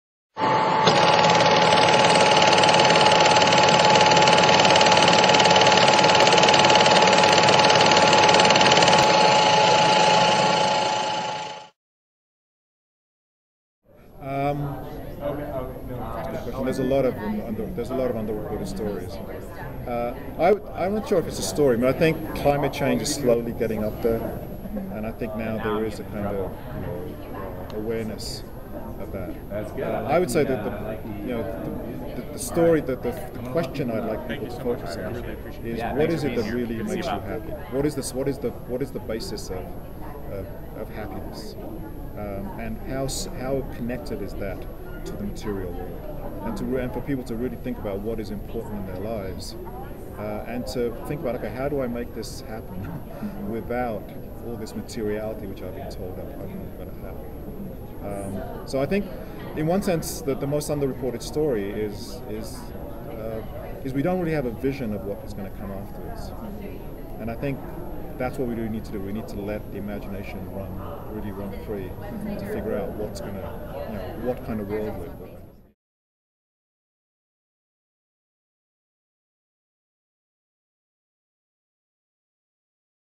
Sut Jhally - What is today's most underreported story?
Sut Jhally, Executive Director of the Media Education Foundation, spoke with the Weave in Denver in April 2013 at the National Conference on Media Reform.